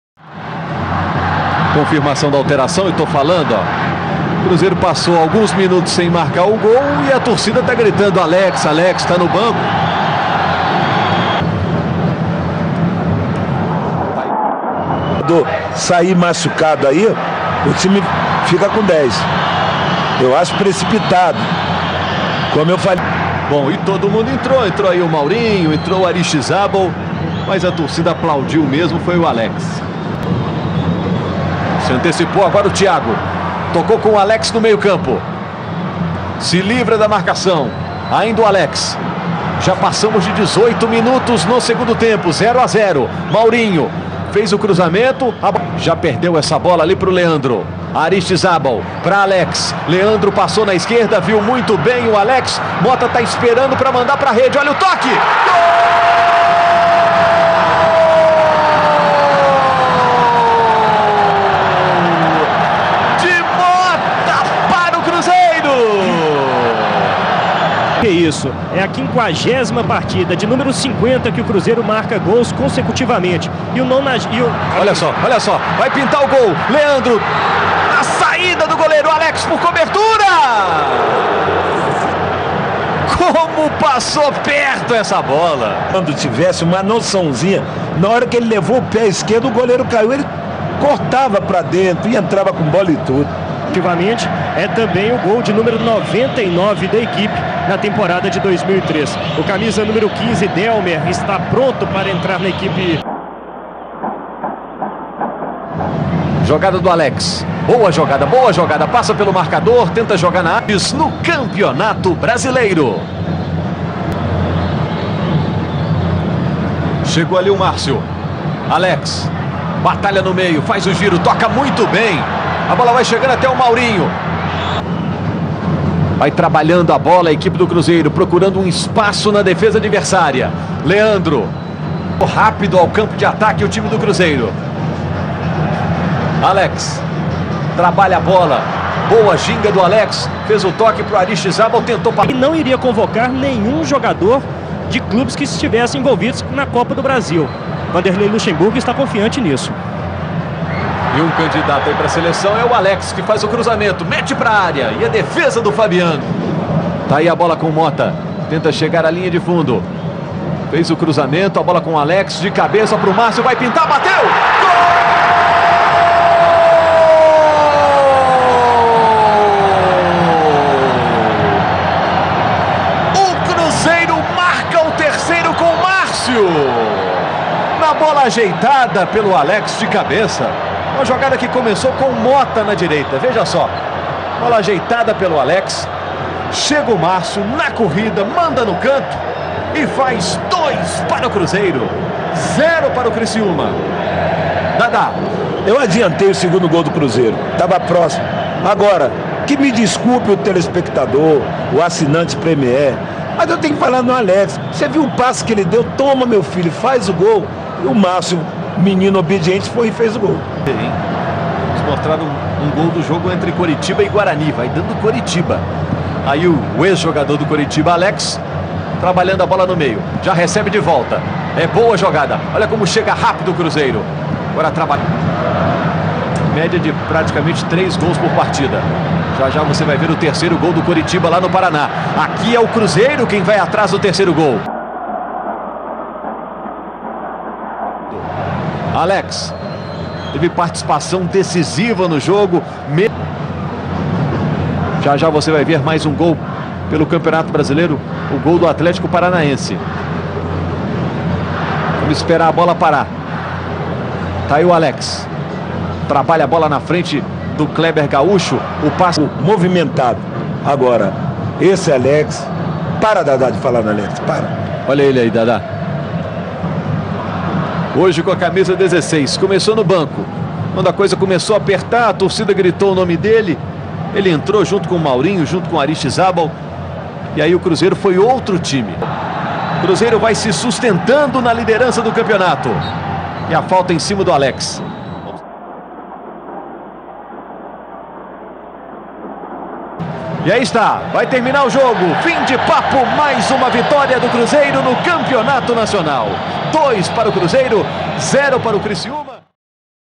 O Cruzeiro não estava jogando nada, até que Alex entrou no jogo...
BrunoA10
A torcida pede e Alex entra no segundo tempo e muda o jogo ajudando o Cruzeiro a sair com mais uma vitória naquele ano.
Cruzeiro 2 x 0 Criciúma campeonato brasileiro 2003
alex talento azul 2003
gols dribles passes
cruzeiro triplice coroa 2003